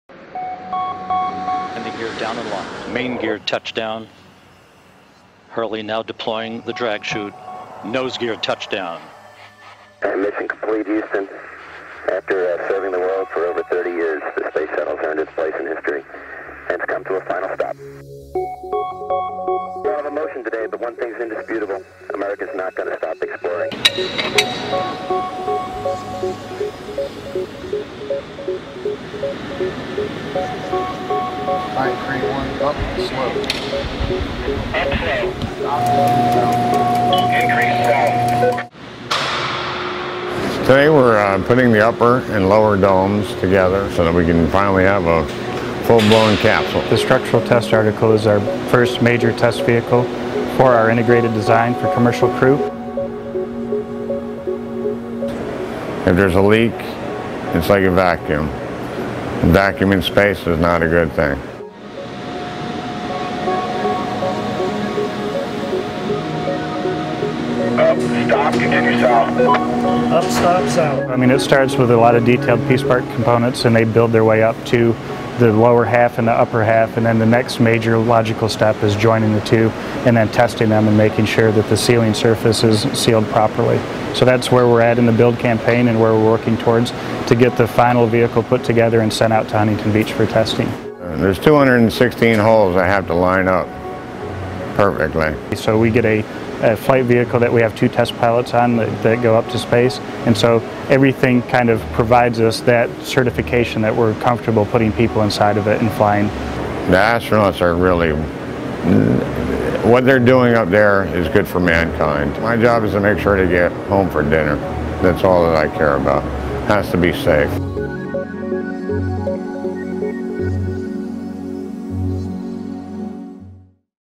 Boeing’s CST-100 Starliner Makes Major Move for U.S. Return to Human Space Flight
In an operation at Kennedy Space Center involving minute movements and precise placement, the pieces of the first CST-100 Starliner test article became a capsule.  The test article will help verify the manufacturing method, the materials, and the parts being created by Boeing and the project’s suppliers and help study the design of the Starliner.